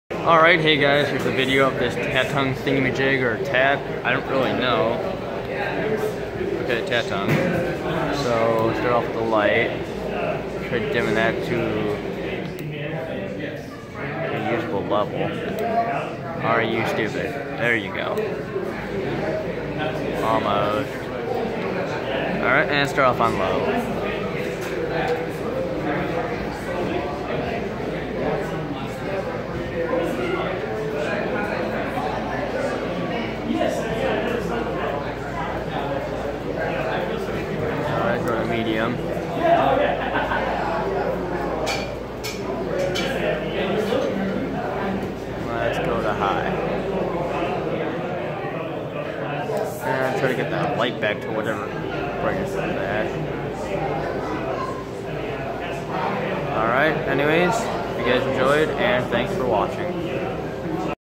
Tatung Lighted Housing ceiling Fan
52" Tatung lighted housing ceiling fan. Very interesting fan. Not to often you see lighted housing fans let alone fans with glass inlay blades. Pretty powerful as well, especially for a hugger.